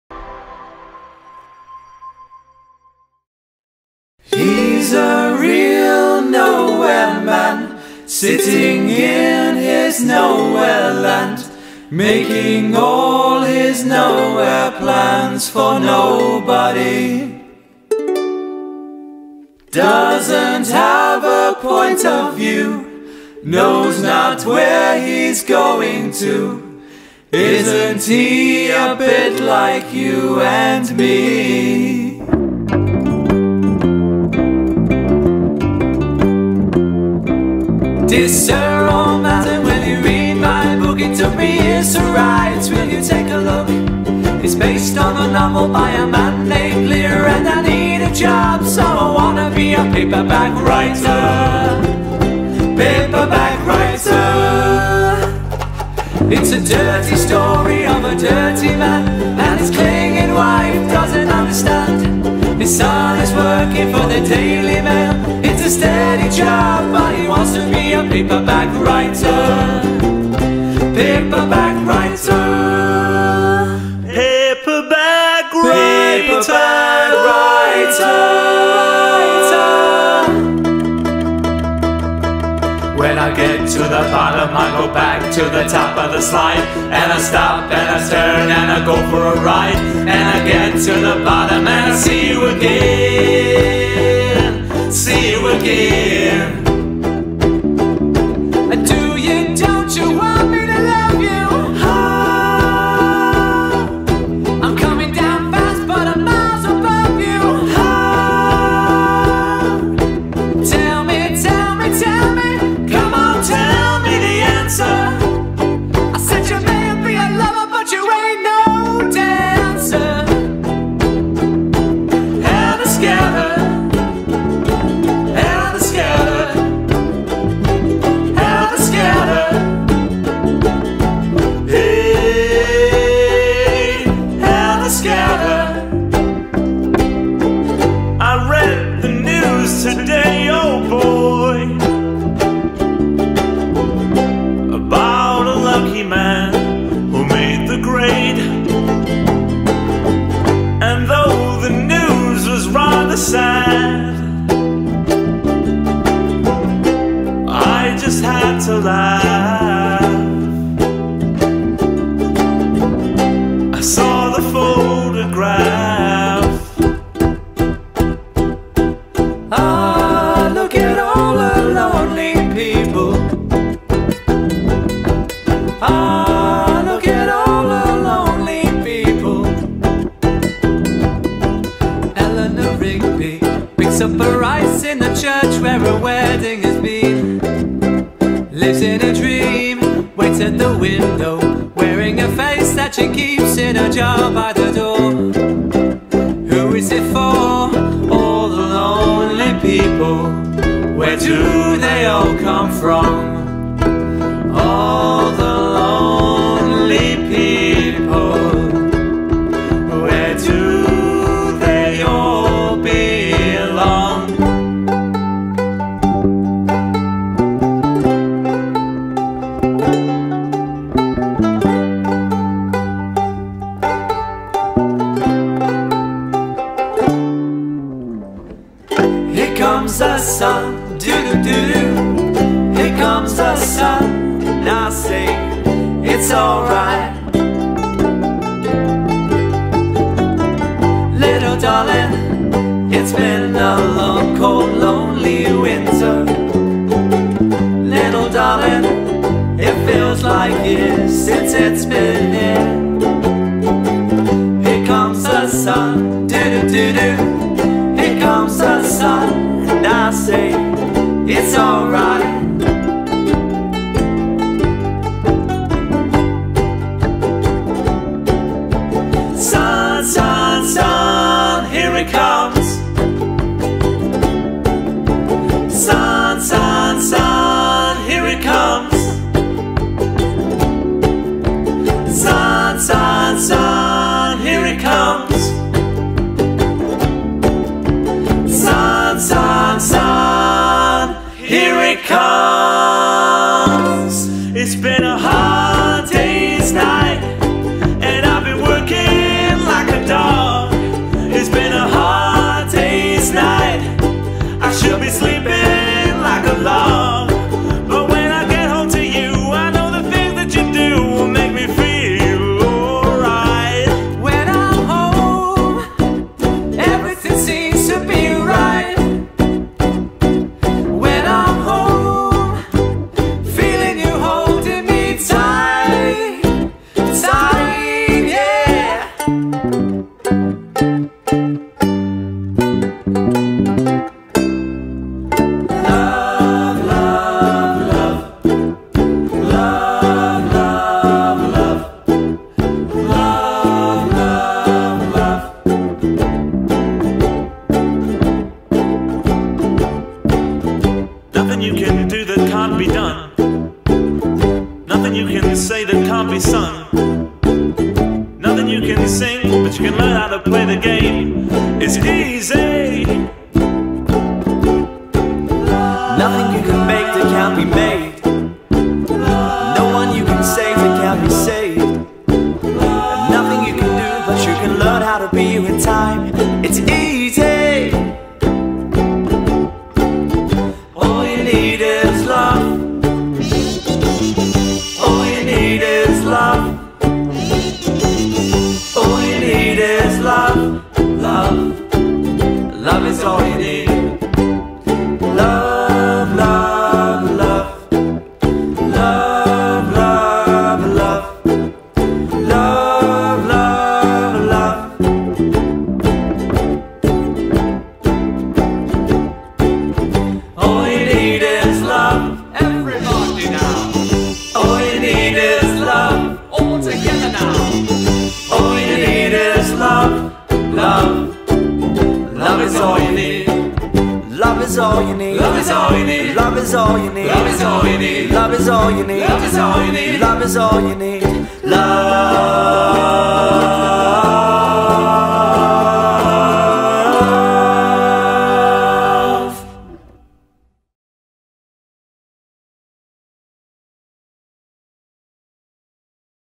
The UkeBoys performing a Beatles Medley - Available from AliveNetwork.com
The UkeBoys are here to completely change the face of the ukulele. An unbelievably entertaining band with bags of experience. From The Beach Boys to Beyonce, The UkesBoys' clever & honed repertoire guarantees to put a smile on everyone's face!